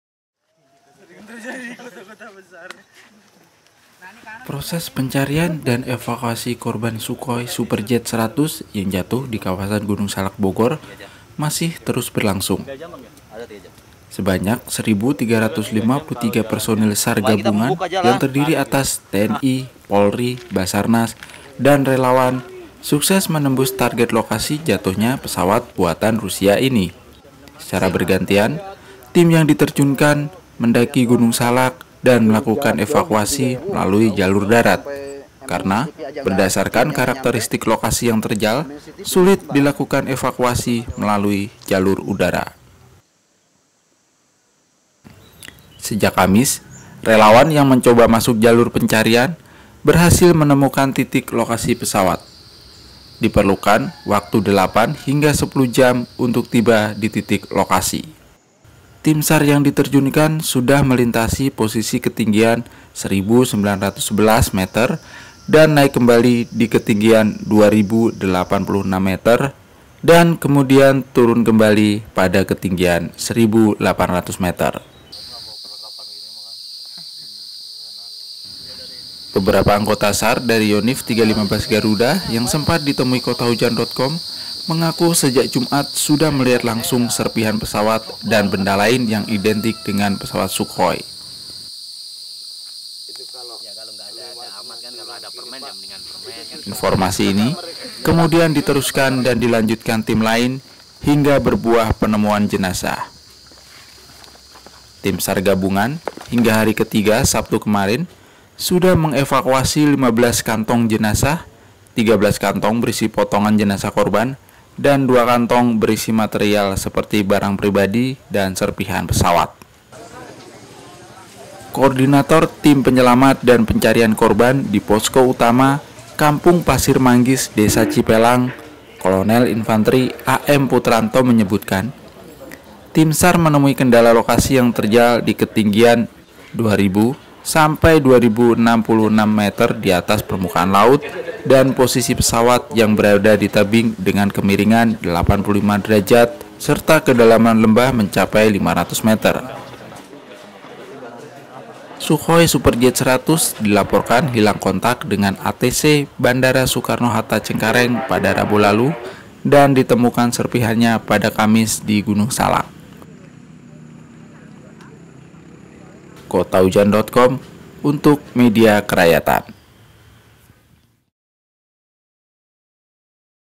Proses Evakuasi Sukhoi, Perjuangan yang Tak mudah
Sejak Kamis (10/5/2012) relawan yang mencoba masuk jalur pencarian berhasil menemukan titik jatuhnya pesawat, diperlukan waktu 8 hingga 10 jam untuk tiba dititik lokasi. Baik dari jalur desa Loji, Cihideung maupun dari posko Desa Cipelang. Tim SAR yang diterjunkan sudah melintasi posisi ketinggian 1.911 meter Gunung Salak dan akan naik kembali ke ketinggian 2.086 meter, kemudian turun lagi ke ketinggian 1.800 meter di mana lokasi jatuhnya pesawat tersebut.